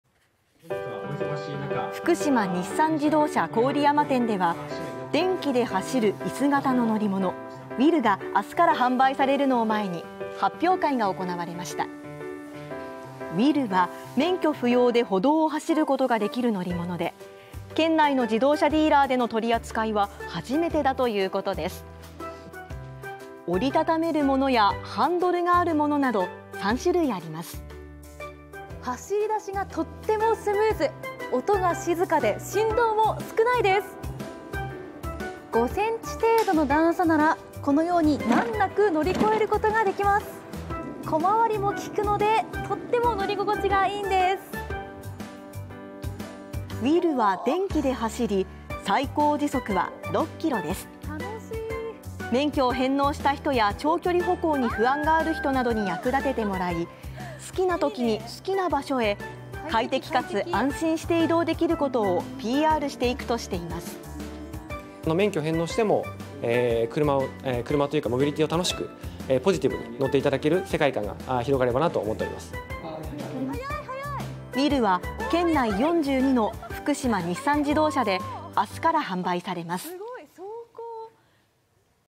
「免許返納後も乗れる」免許不要で歩道を走る「WHILL（ウィル）」販売 福島日産自動車が県内初
福島県郡山市の自動車ディーラーでは、免許不要で歩道を走ることができる乗り物の発表会が行われました。福島日産自動車・郡山店では、電気で走るイス型の乗り物「WHILL（ウィル）」が24日から販売されるのを前に、発表会が行われました。「WHILL」は免許不要で歩道を走ることができる乗り物で、県内の自動車ディーラーでの取り扱いは初めてだということです。折りたためるものやハンドルがあるものなど、3種類あります。安部遼アナウンサー「走り出しがスムーズ！音が静かで振動も少ないです。5センチ程度の段差なら、このようになんなく乗り越えることができます。小回りも効くのでとても乗り心地が良いです。」「WHILL」は電気で走り、最高時速は6キロです。免許を返納した人や、長距離歩行に不安がある人などに役立ててもらい、好きなときに好きな場所へ快適かつ安心して移動できることをPRしていくとしています。福島日産自動車・金子與志幸社長「免許を返納しても、モビリティ（乗り物）を楽しくポジティブに乗っていただける世界観が広がればなと思っています。」「WHILL」は県内42の福島日産自動車で、24日から販売されます。